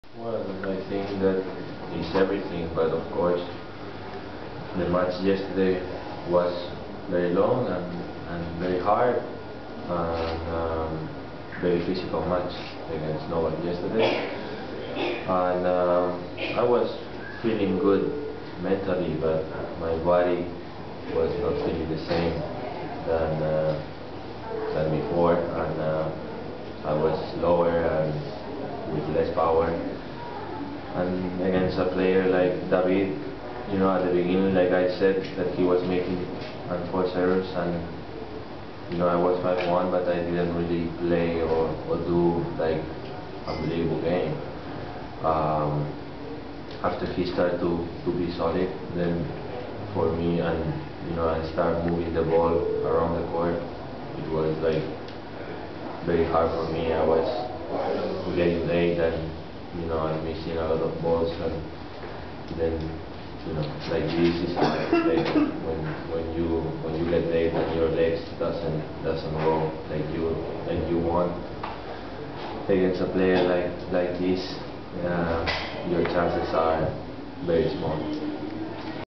Roma 2010 - Verdasco in conferenza 2
Roma, 1 maggio 2010, Fernando Verdasco in conferenza stampa dopo la sconfitta in semifinale contro David Ferrer